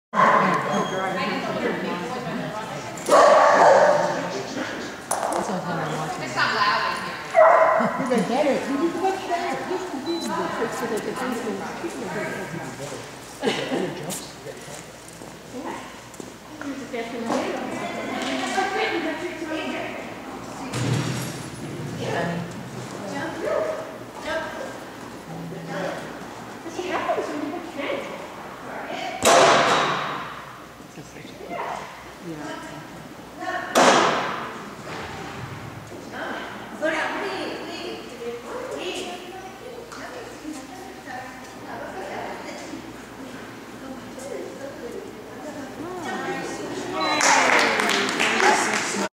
Allison Henson Standard KK DOCNA Oct 2012
Allison and Henson doing a DOCNA standard agility course at the Kruisin Kanines DOCNA trial.